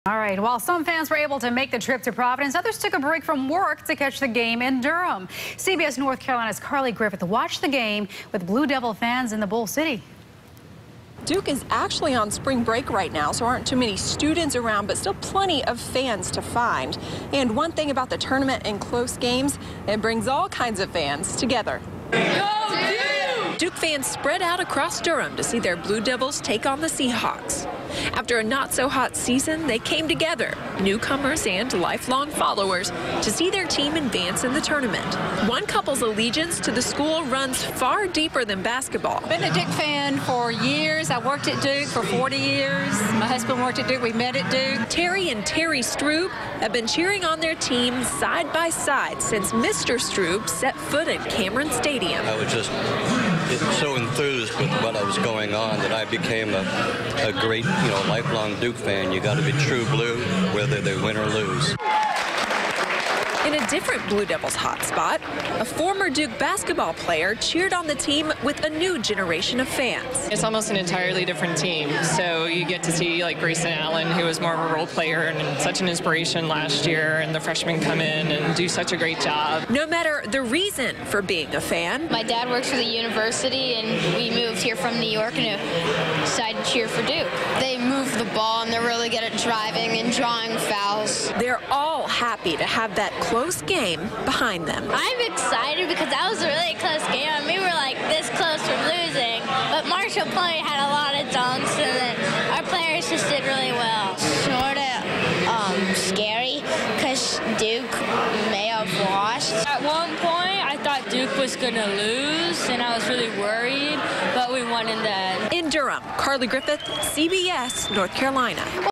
Duke fans cheer on Blue Devils in NCAA Tournament
The Duke Blue Devils took on the UNC Wilmington Seahawks on the first day of the NCAA Tournament.